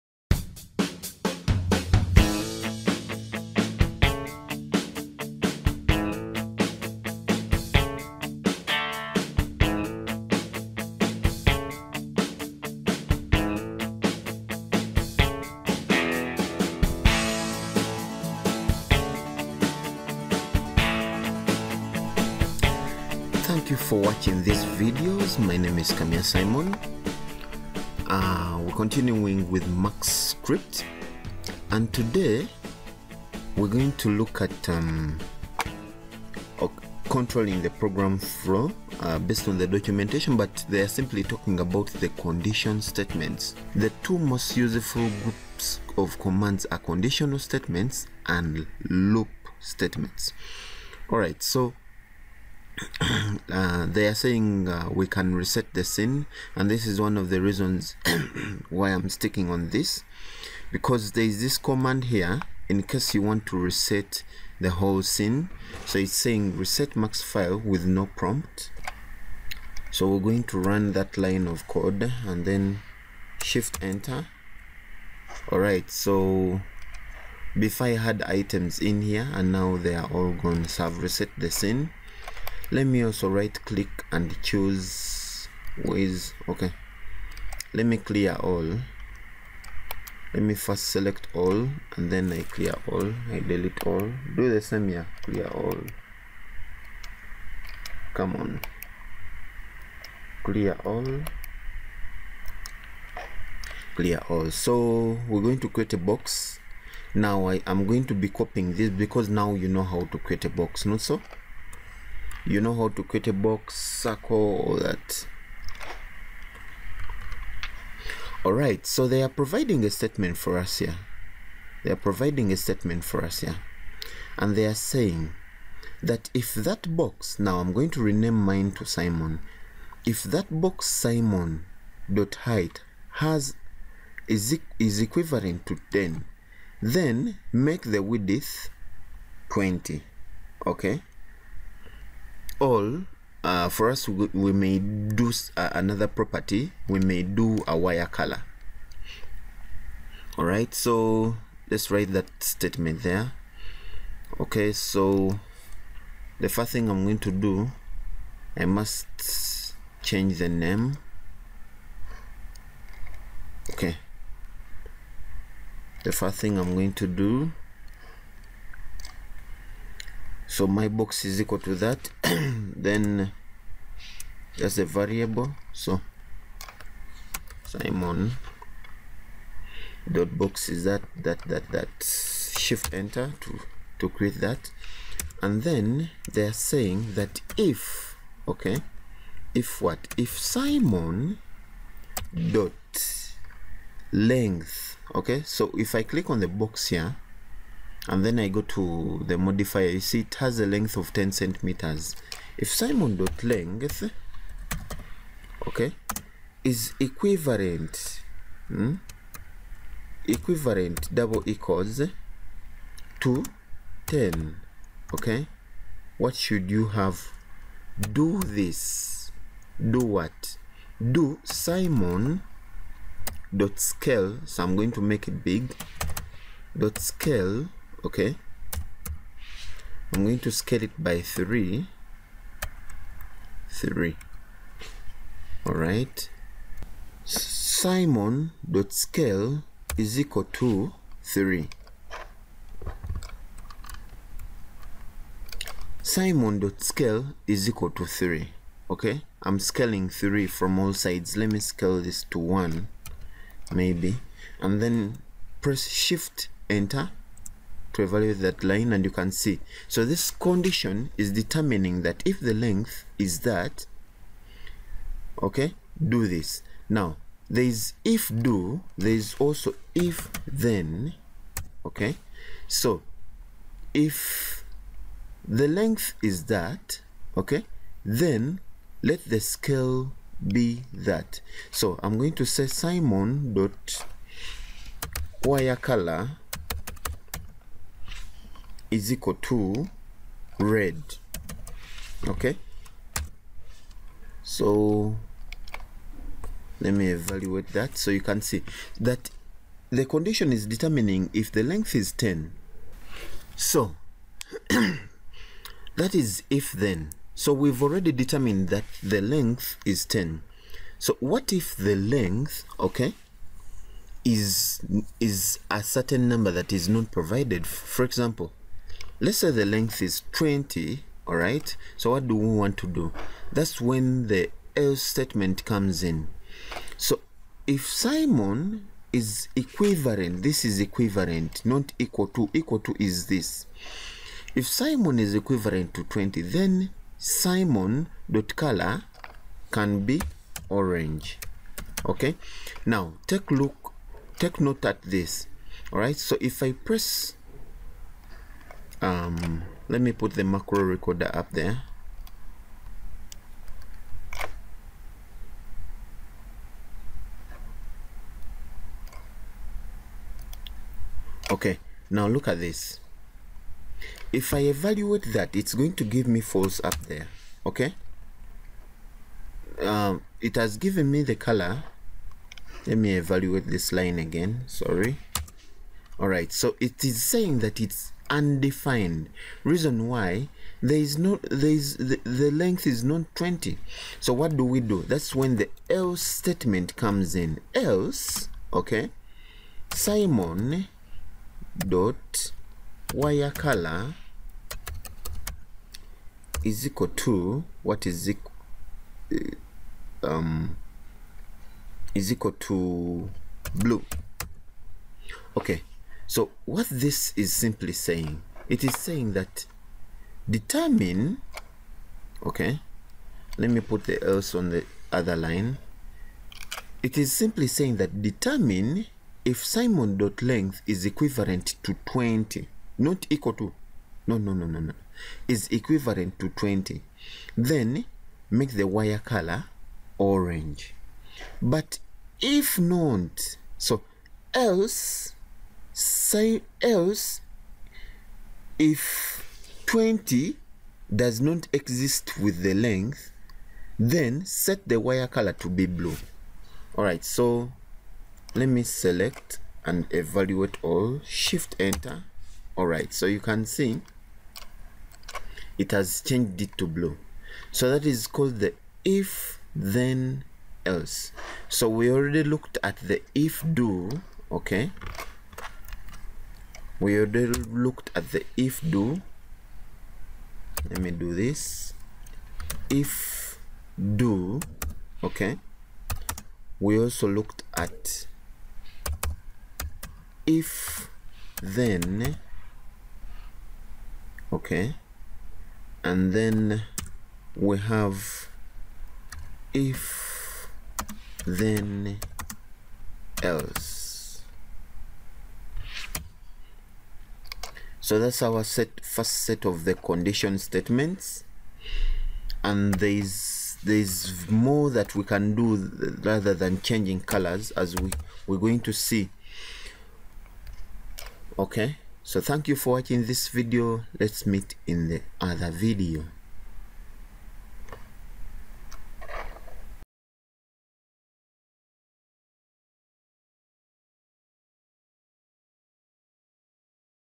Maxscript 12_ If,do,then,else Conditions
skype id
In this lesson we learn how to work around the basic condition statement as shall be use for our project script.